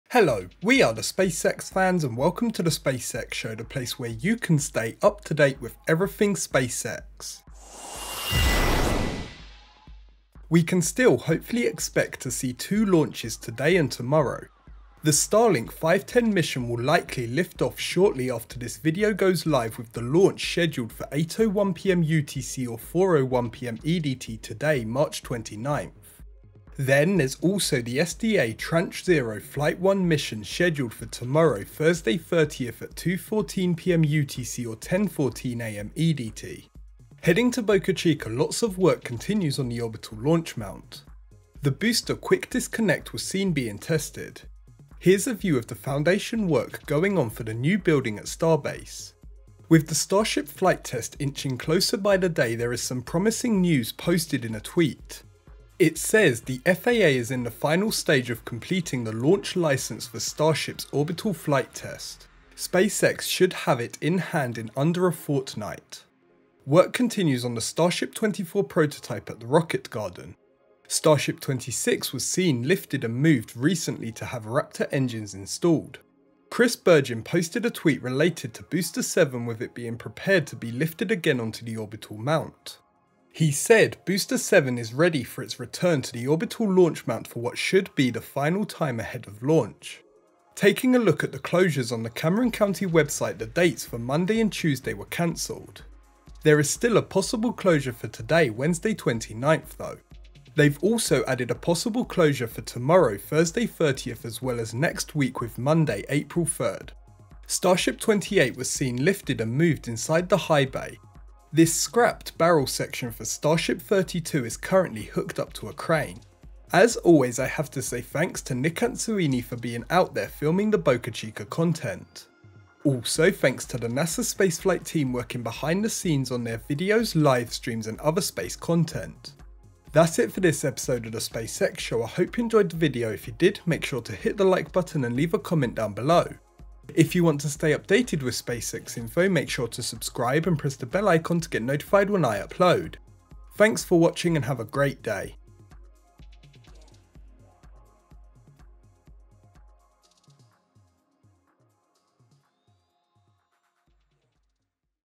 SpaceX Starship Updates! Booster 7 Preparing For Lift Back Onto Orbital Mount! TheSpaceXShow
Welcome to the latest SpaceX Starship updates video from TheSpaceXFans. We have a couple of launch related bits of information for the standard SpaceX news. Over in Boca Chica, as well as work continuing at the orbital launch site, there is also Booster 7 preparing for lift back onto orbital mount! Plus, in the production yard, work continues on future prototypes. Sit back and enjoy!

Nic Ansuini & NSF Robotic Camera. Booster Quick Disconnect Tested Before Launch | SpaceX Boca Chica. YouTube. NASASpaceflight. March 29, 2023.